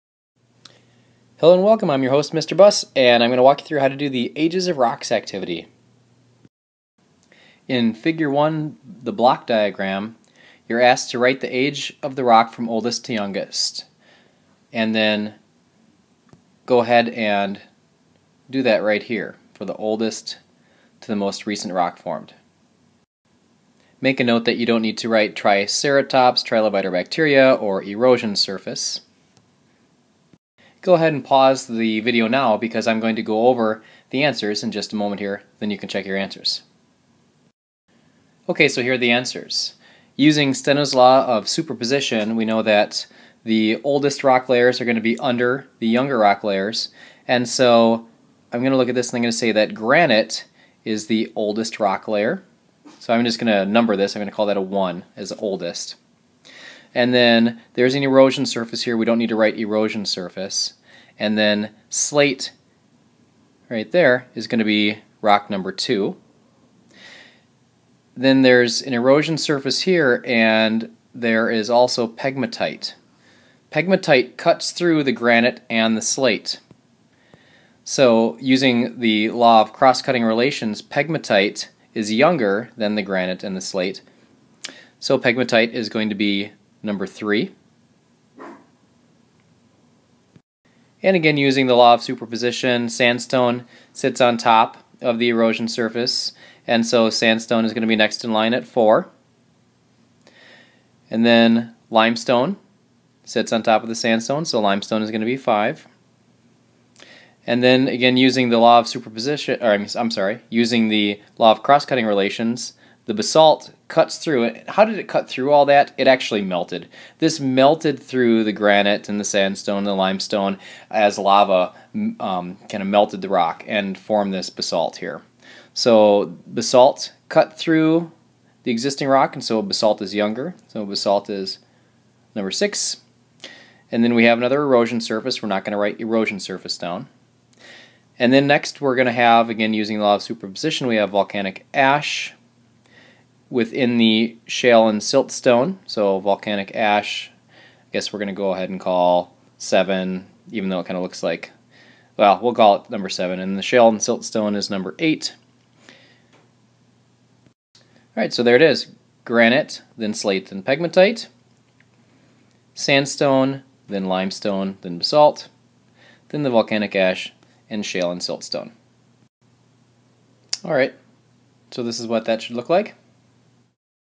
Lab 1 Ages of Rocks Part 1
Farmington High School Mr. Buss Earth Science Fall 2013 Ages of Rocks Activity Part 1